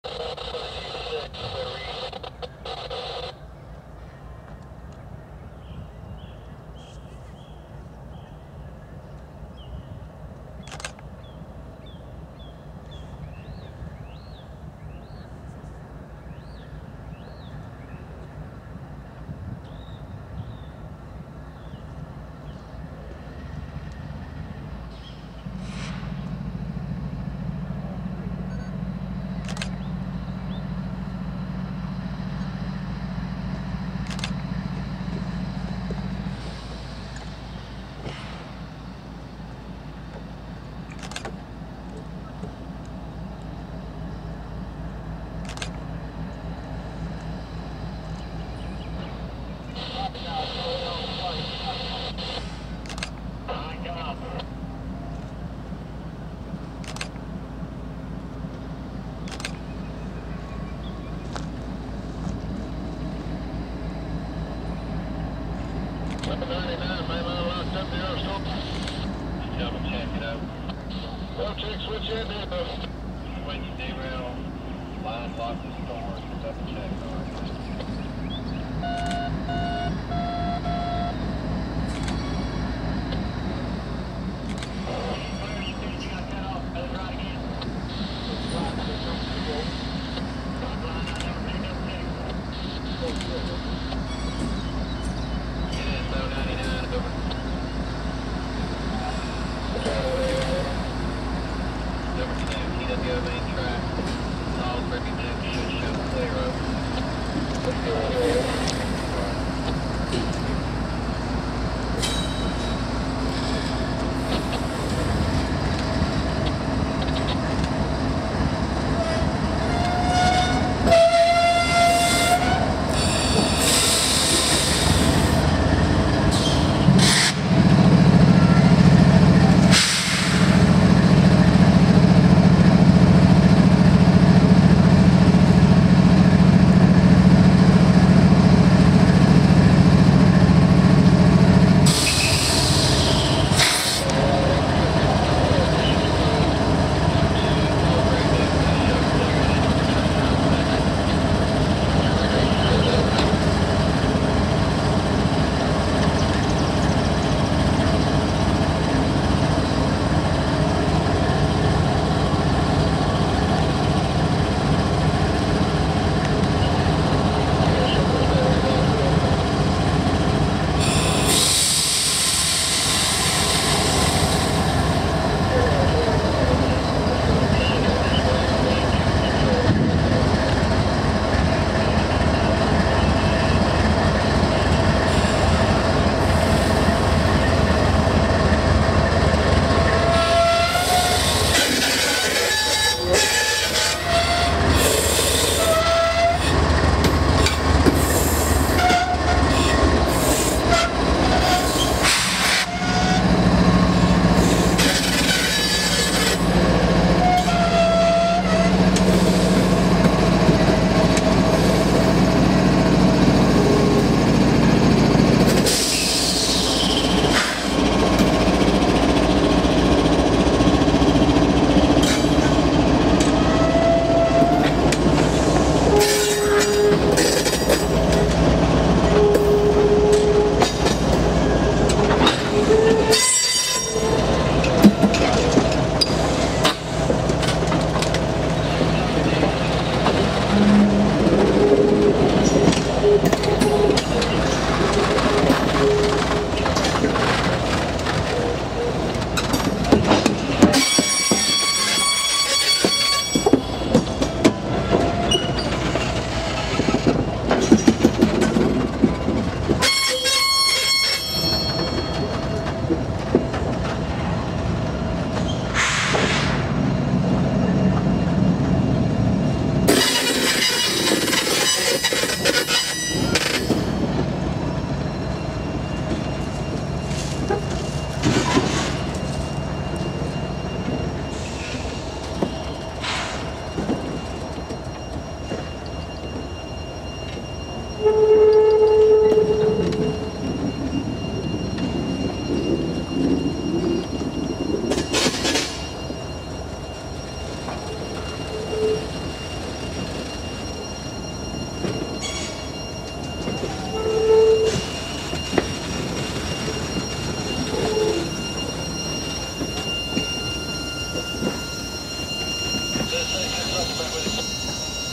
Norfolk & Western 611 Arrives at the NC Transportation Museum
Norfolk Southern 8103 leads the Wabash 1089 and the Norfolk & Western 611 on to the museum tracks at the North Carolina Transportation Museum in Spencer, North Carolina.